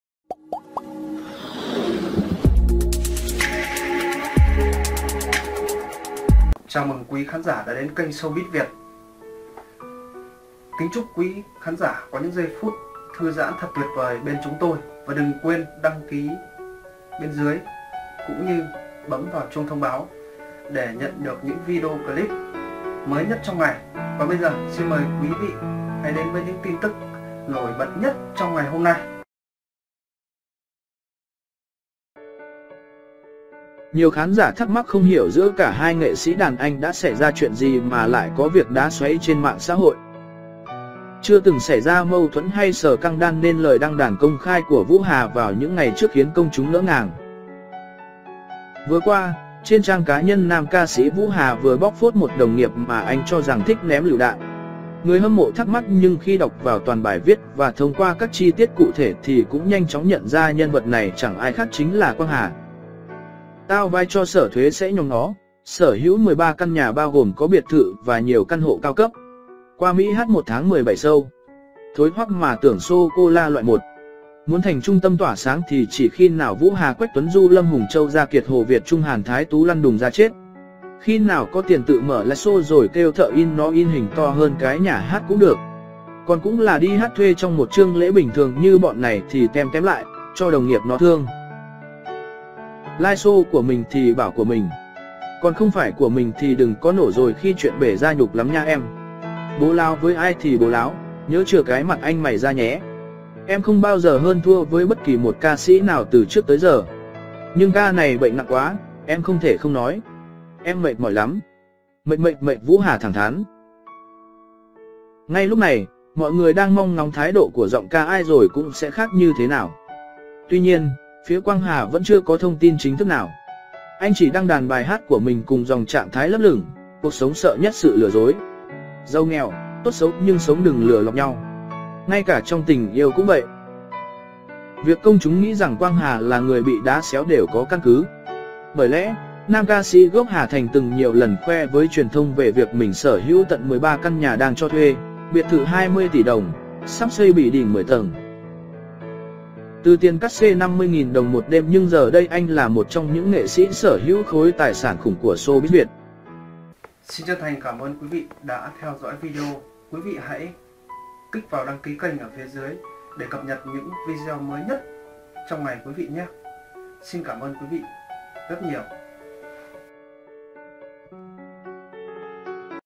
Quang Hà lấp lửng khi bị Vũ Hà “cà khịa”: “Bớt ném lựu đạn giùm cái”
Nhiều khán giả thắc mắc không hiểu giữa cả hai nghệ sĩ “đàn anh” đã xảy ra chuyện gì mà lại có việc “đá xoáy” trên mạng xã hội.